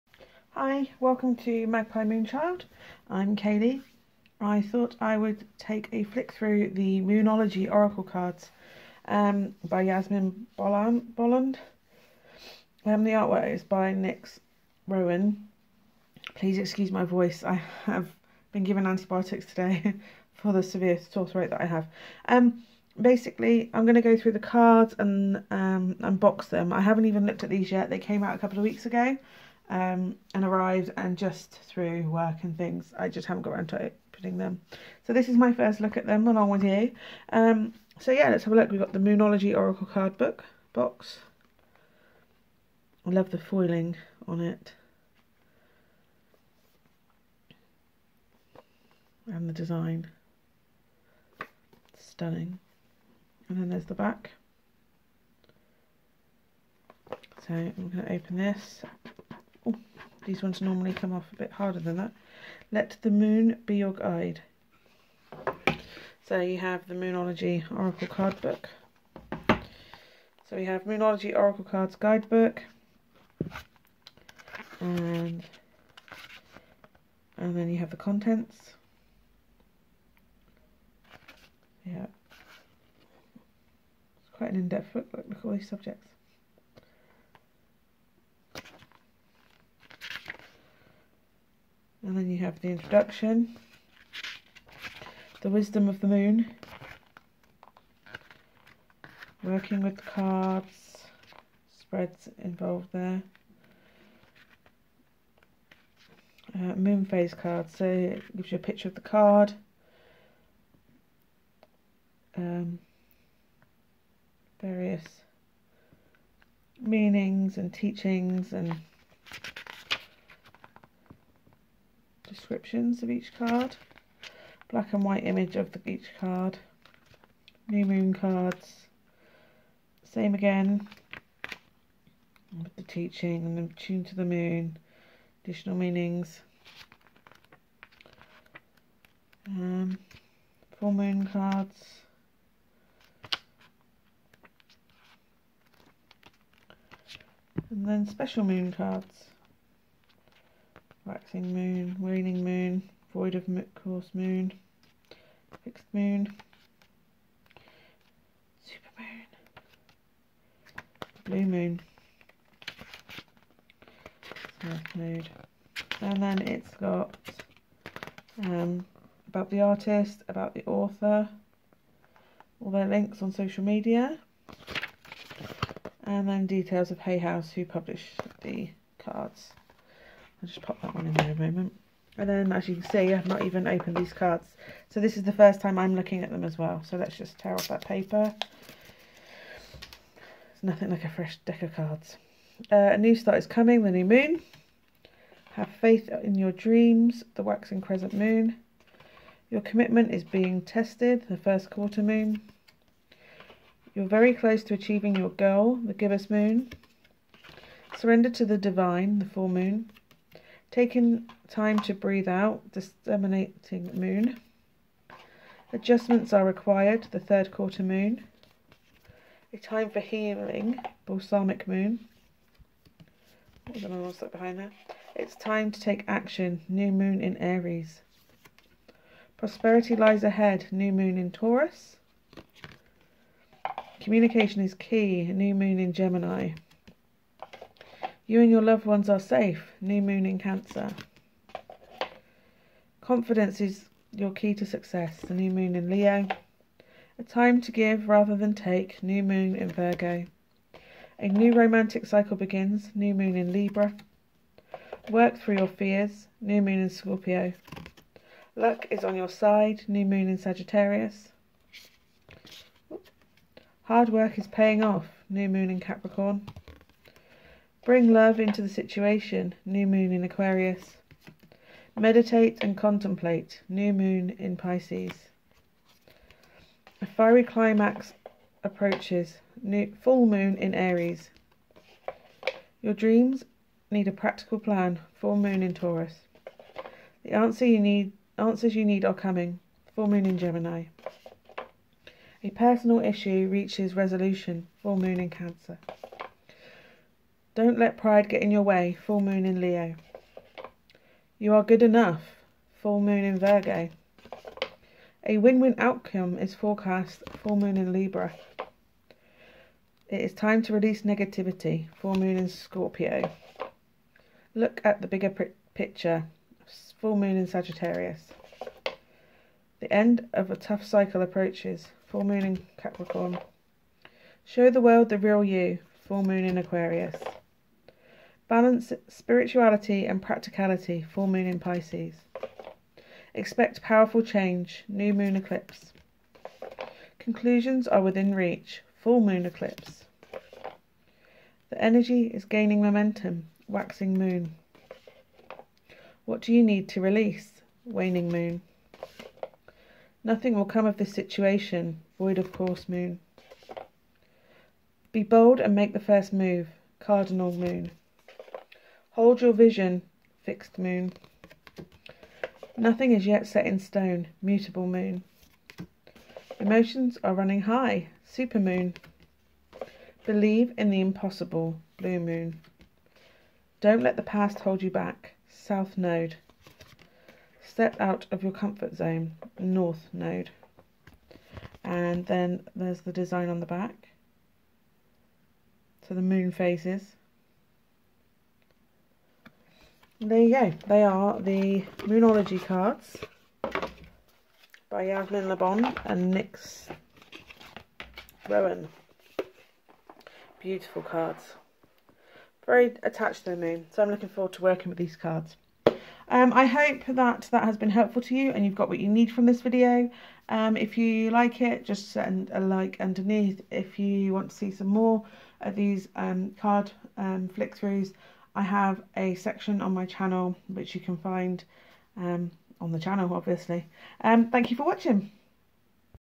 Moonology Oracle Card Deck by Yasmin Boland and Nyx Rowan - Deck Flip Thru
A card by card flick through of the Moonology Oracle Deck from Yasmin Boland and Nyx Rowan

Hope this video gives you what you need from it.

Kayleigh aka Magpie Moonchild